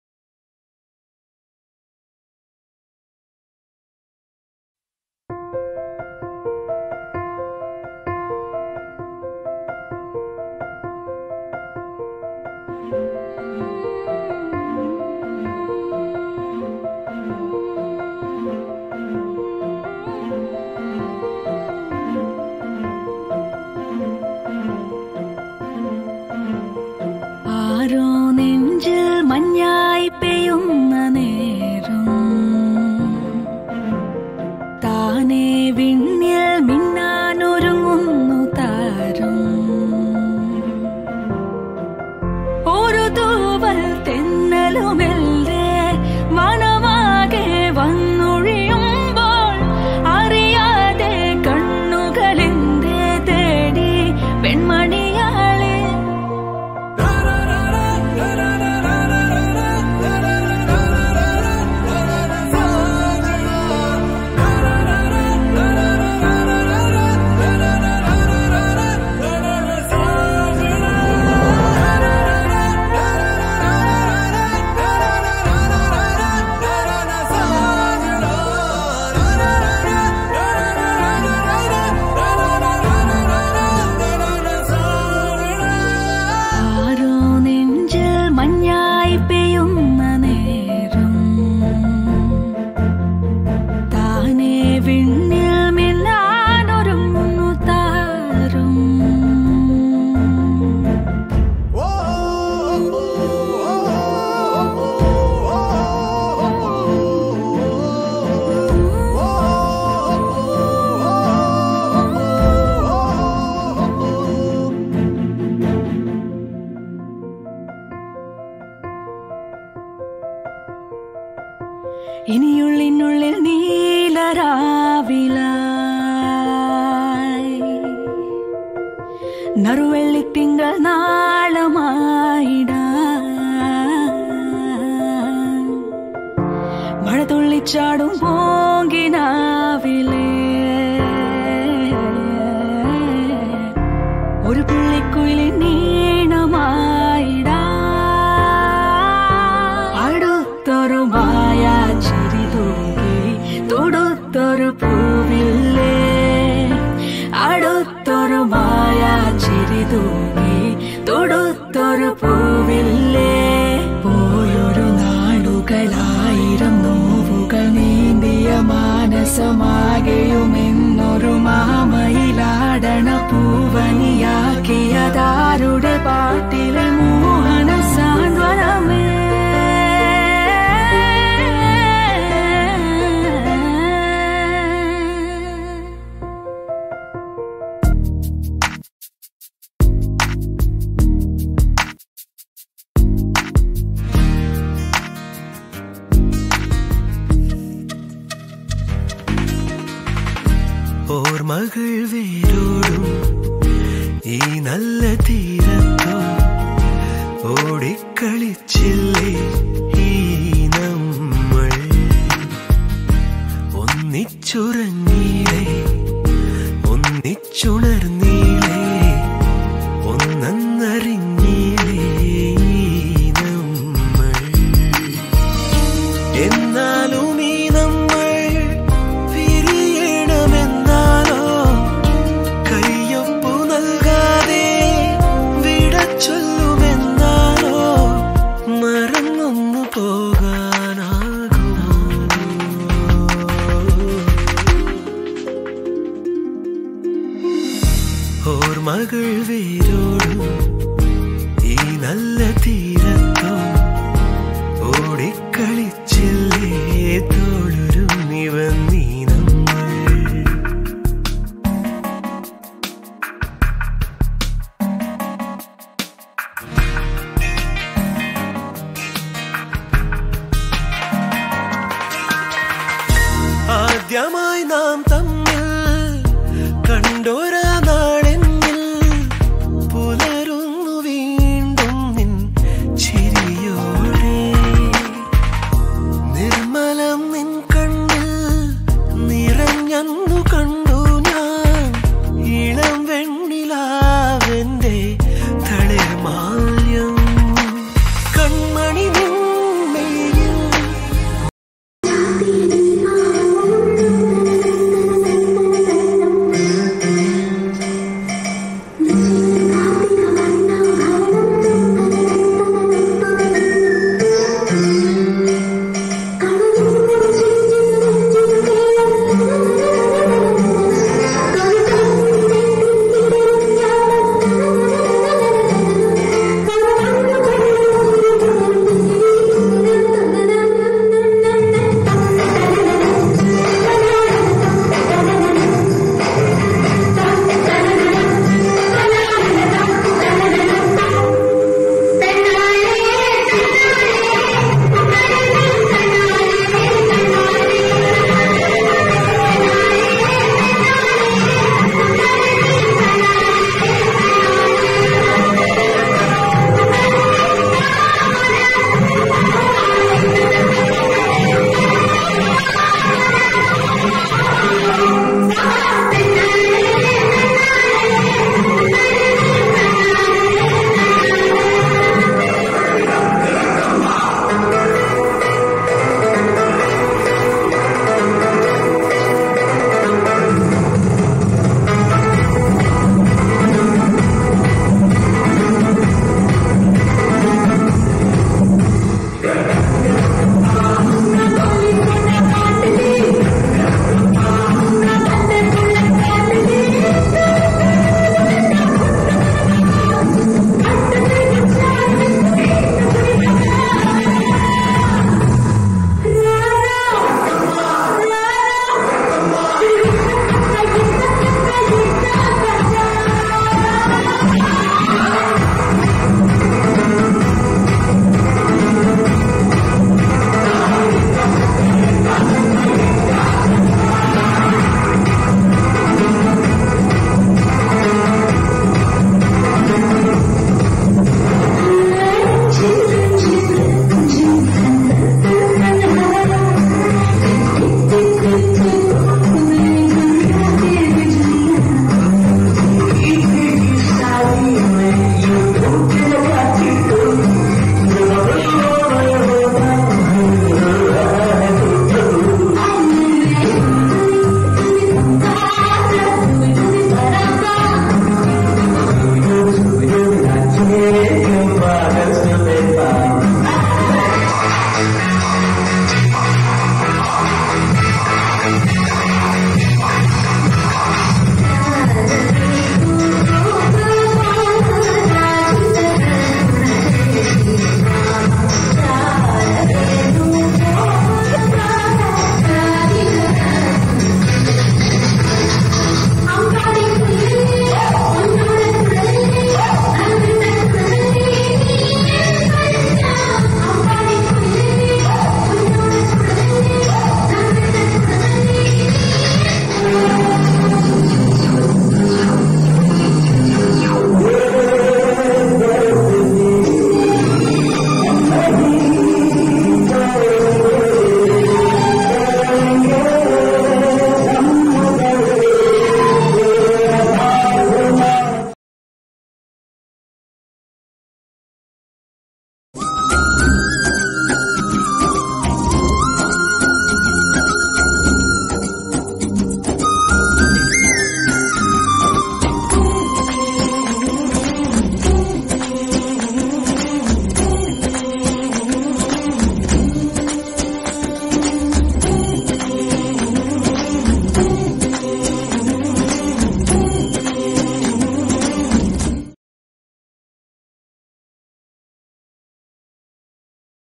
FAREWELL TO OUR DEAR CHUSHI TEACHER &MINI TEACHER@rmhighersecondaryschoolalo7028
farewell party for our dears
best wishes for their upcoming future @rmhighersecondaryschoolalo7028